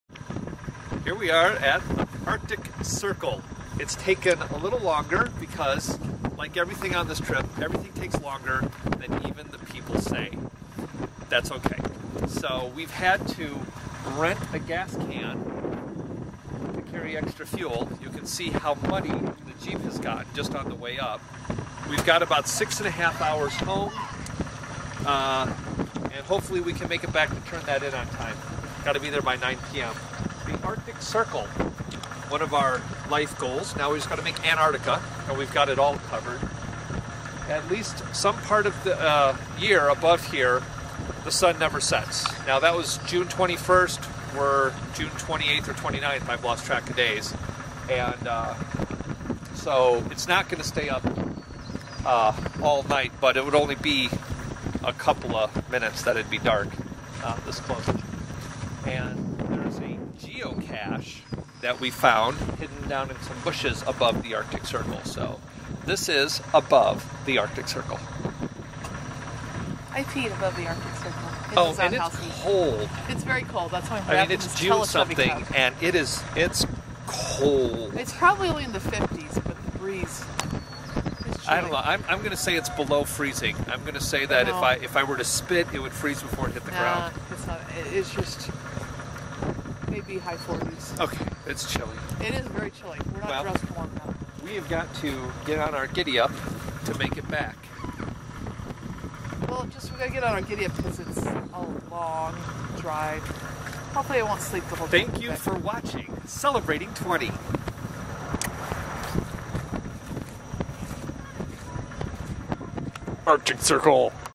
Canada Alaska.m4v
Arctic Circle using the Dempster highway in Yukon Canada